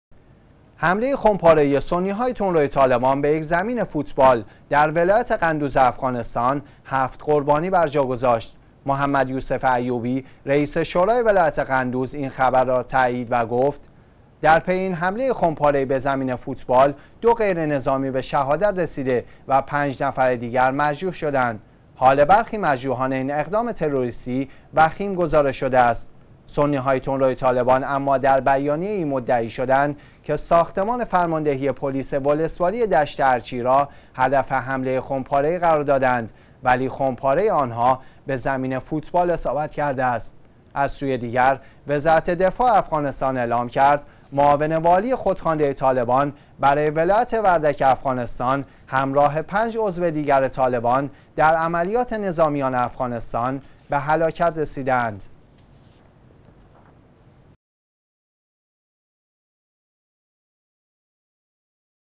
حمله طالبان به یک زمین ورزشی
حمله خمپاره‌ای سنی های تندروی طالبان به یک زمین فوتبال در ولایت قندوز افغانستان 7قربانی بر جا گذاشت.
«محمد یوسف ایوبی» رئیس شورای ولایت قندوز این خبر را تایید کرد و گفت: در پی این حمله خمپاره ای به زمین فوتبال، 2غیر نظامی به شهادت رسیده و 5 نفر دیگر مجروح شدند.
حال برخی مجروحان این اقدام تروریستی وخیم گزارش شده است.
سنی های تندروی طالبان اما در بیانیه ای مدعی شدند که ساختمان فرماندهی پلیس ولسوالی دشت ارچی را هدف حمله خمپاره‌ای قرار داند، ولی خمپاره آن‌ها به زمين فوتبال اصابت کرده است.
از سوی دیگر وزارت دفاع افغانستان اعلام کرد: معاون والی خودخوانده طالبان برای ولایت وردک افغانستان همراه پنج عضو دیگر طالبان در عملیات نظامیان افغانستان به هلاکت رسیدند.